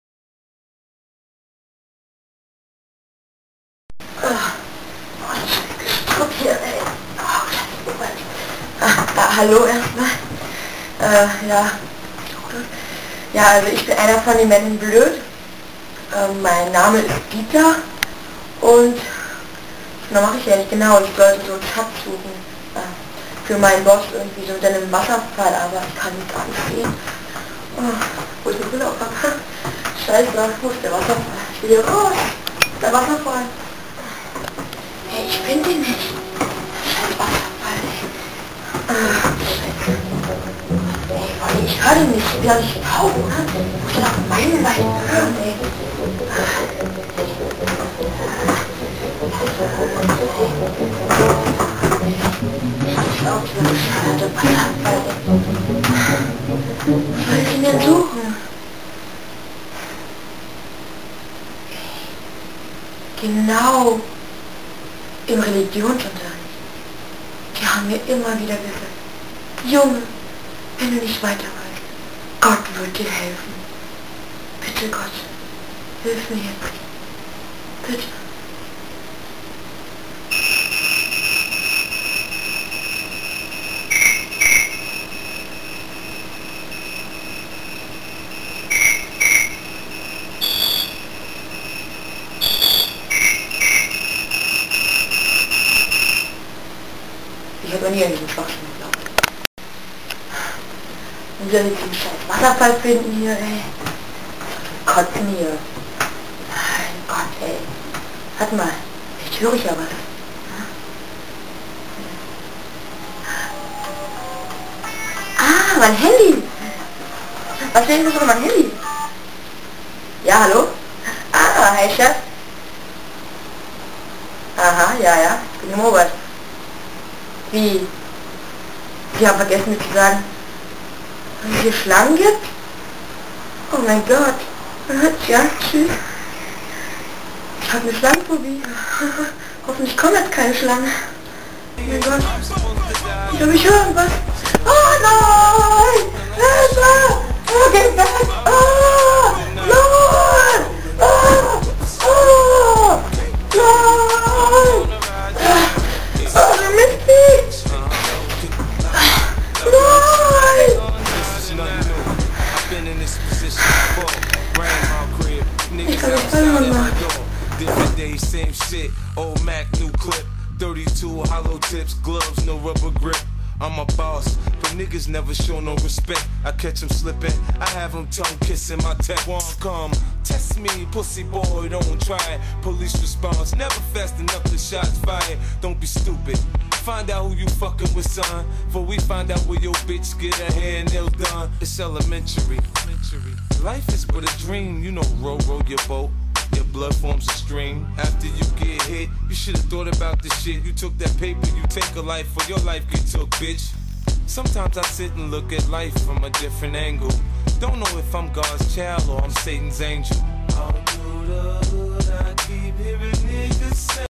Men in bloed (german)
A video about Diedder, a special agent of the men in bloed.